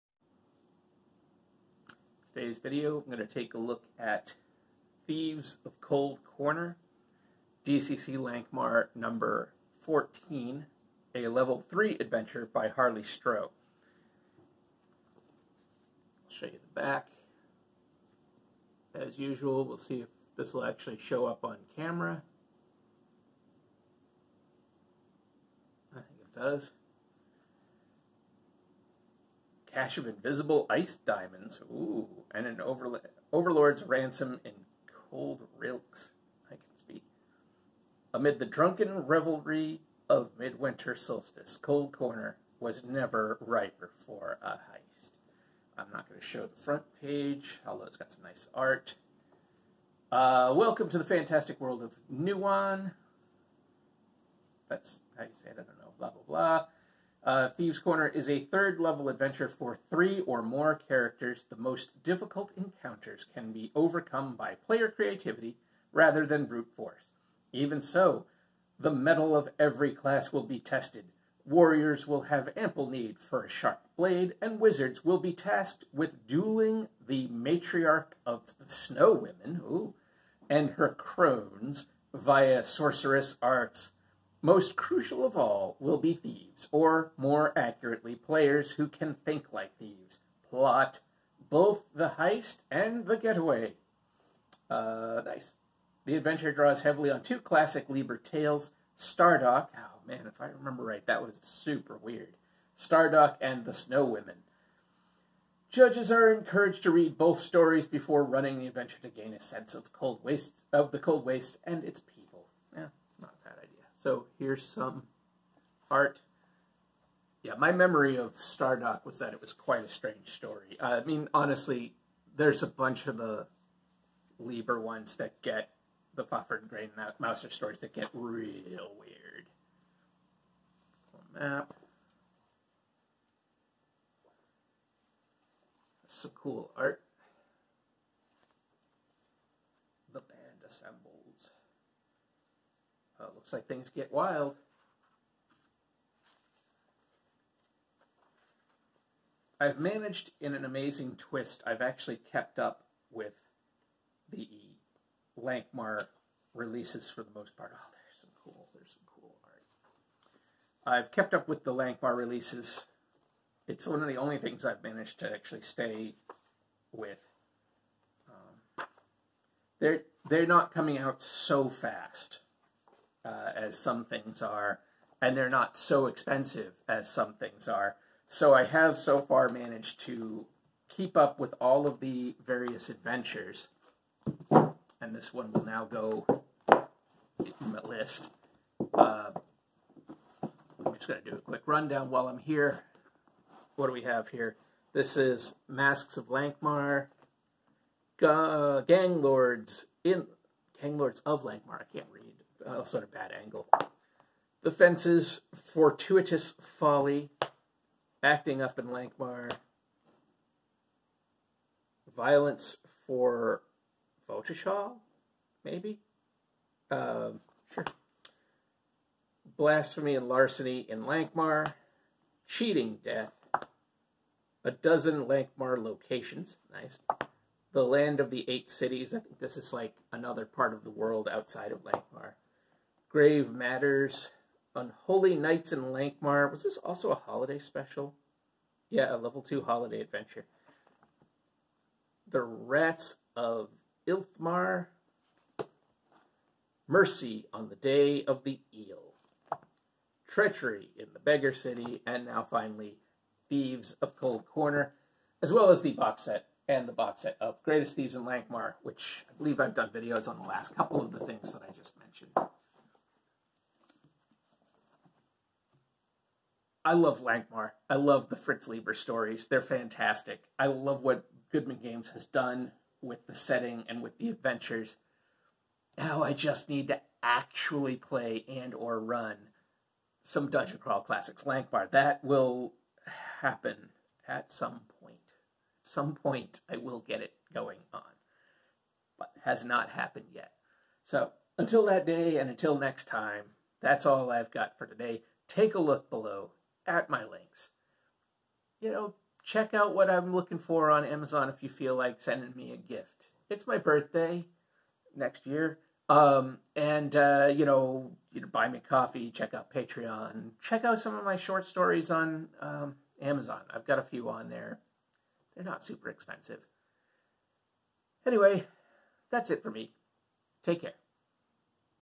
First Look at Thieves of the Cold Corner (DCC)
Hi!  I'm Matt.  I'm an independent author, movie buff, intrepid traveler, and tabletop RPG enthusiast.

Amazon Wishlist

Video Notes

In this video, I take a look at Thieves of the Cold Corner, a level 3 adventure for Dungeon Crawl Classics Lankhmar by Harley Stroh.